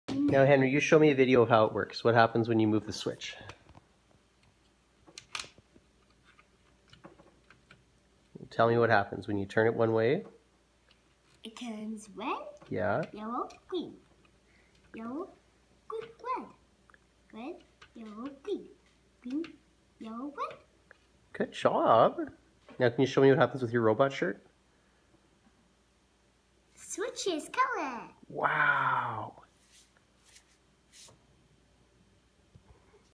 Henry playing with his Circuit Maze
Henry loves to play with his Circuit Maze, especially with his cool robot shirt